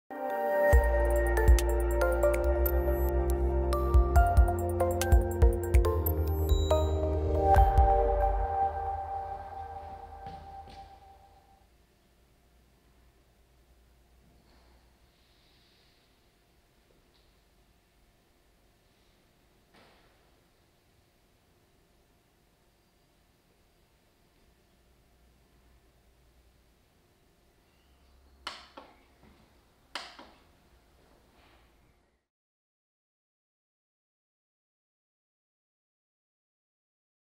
burgbad Bedienung Spiegelschrank LED Sensorschalter Lichttemperatur Steuerung 1
In diesem Video zeigen wir Ihnen die Bedienung des Spiegelschranks mit Sensorschalter und Lichttemperatur-Steuerung.